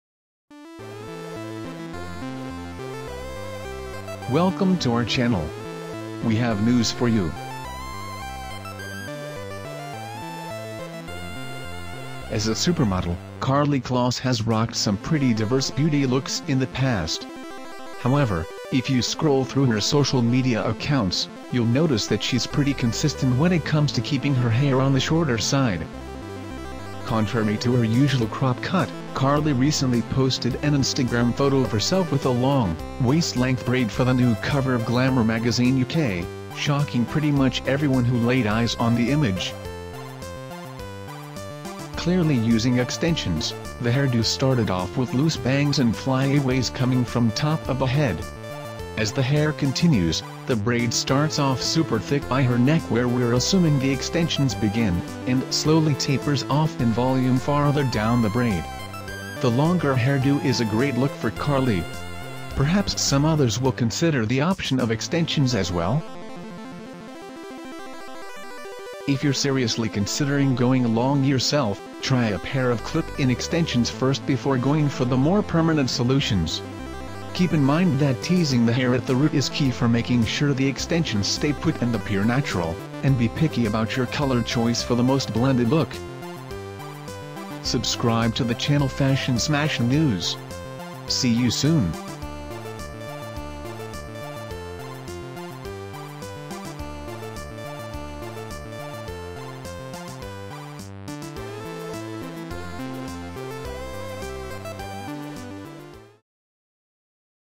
We've Never Seen Karlie Kloss With Hair This Long Before
As a supermodel, Karlie Kloss has rocked some pretty diverse beauty looks in the past. However, if you scroll through her social media accounts, you’ll notice that she’s pretty consistent when it comes to keeping her hair on the shorter side. Contrary to her usual cropped cut, Karlie recently posted an Instagram photo of herself with a long, waist-length braid for the new cover of Glamour Magazine UK, shocking pretty much everyone who laid eyes on the image.

Image courtesy of Instagram/Karlie Kloss